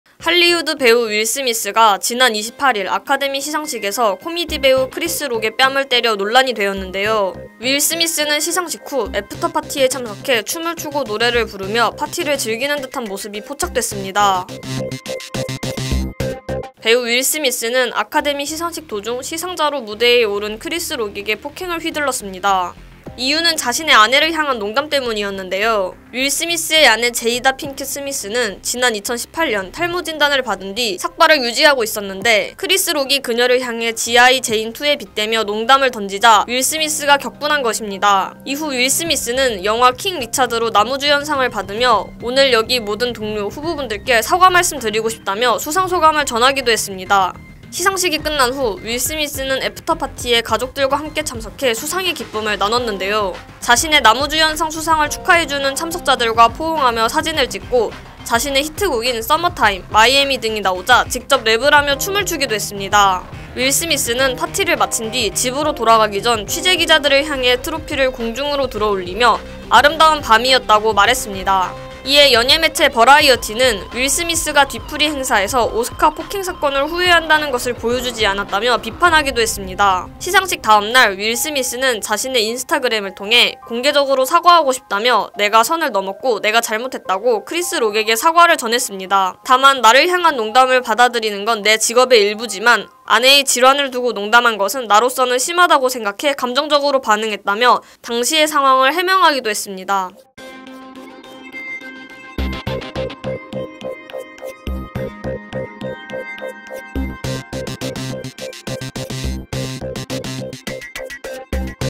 뺨 때리고 춤 추고...윌스미스 반성은 하니?
(서울=뉴스1) 엄예진 인턴기자 = 할리우드 배우 윌 스미스가 지난 28일 아카데미 시상식에서 코미디 배우 크리스 록의 뺨을 때려 논란이 되었는데요.

윌 스미스는 시상식 후 애프터 파티에 참석해 춤을 추고 노래를 부르며 파티를 즐기는 듯한 모습이 포착됐습니다.

자세한 내용은 영상에서 확인하세요.